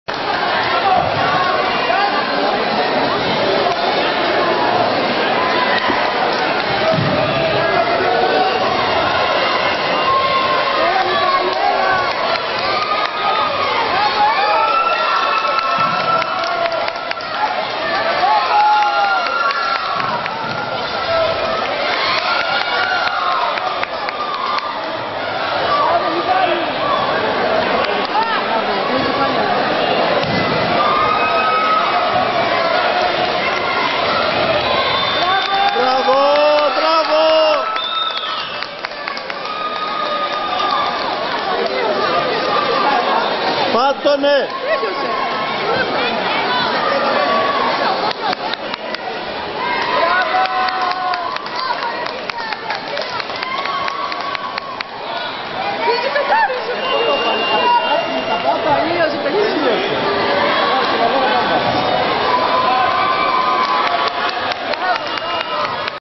Tae Kwon Do kampitakis michalis (Καμπιτάκης Μιχάλης)
Διασυλλογικοί Αγώνες Tae Kwon Do Ανω Λιόσια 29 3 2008 Συμετοχή με Α.Σ.Μοιρών Μιχάλης Καμπιτάκης.